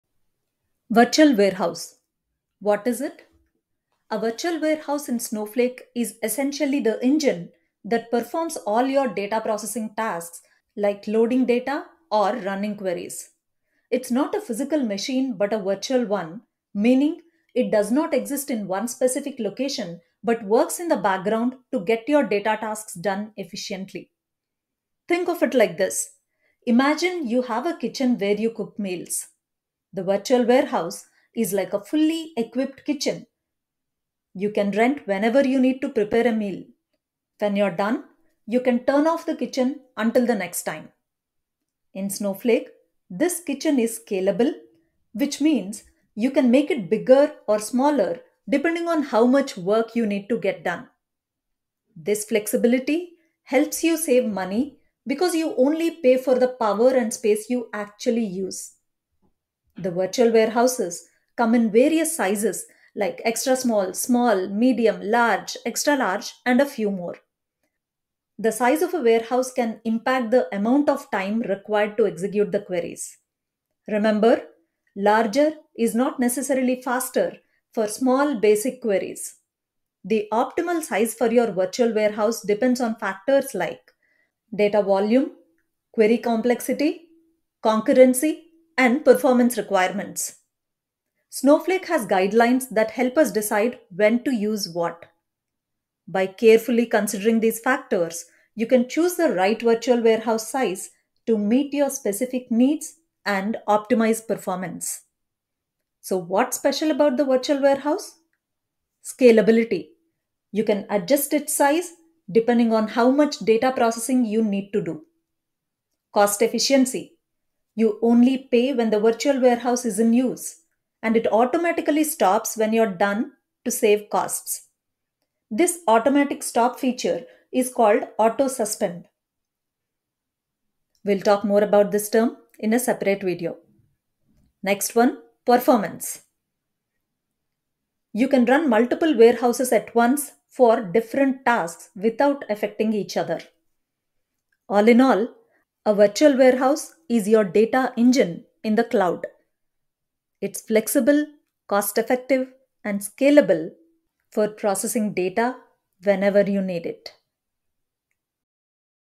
What is Virtual Warehouse in Snowflake ❄️?
🤔 Understand the significance of Virtual Warehouse in Snowflake ❄️

Share your thoughts, questions, and experiences in the comments section below. Let's build a community of data enthusiasts!